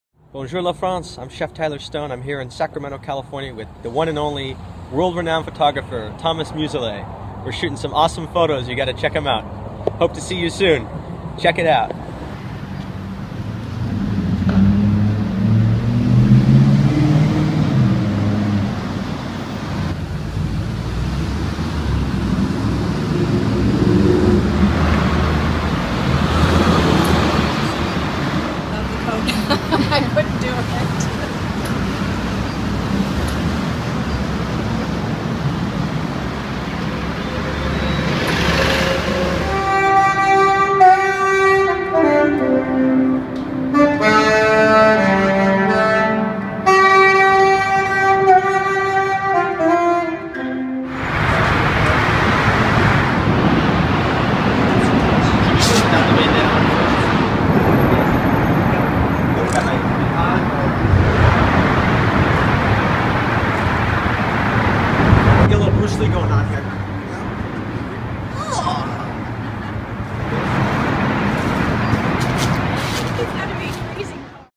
Shooting California Sacramento with the Chef Tyler stone / Thomas Muselet Photography
Making of Shooting Thomas Muselet In Sacamento California with the Famous Chef Tyler Stone.